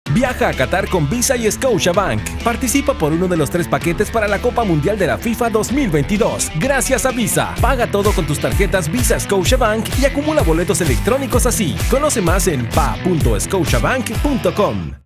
Ve a Catar 2022™ con Scotiabank y Visa
Aún estás a tiempo de ganar un viaje y $5k en tu tarjeta de crédito para vivir la fiesta del fútbol en Catar 2022™, acumula boletos electrónicos con tus compras. Conoce más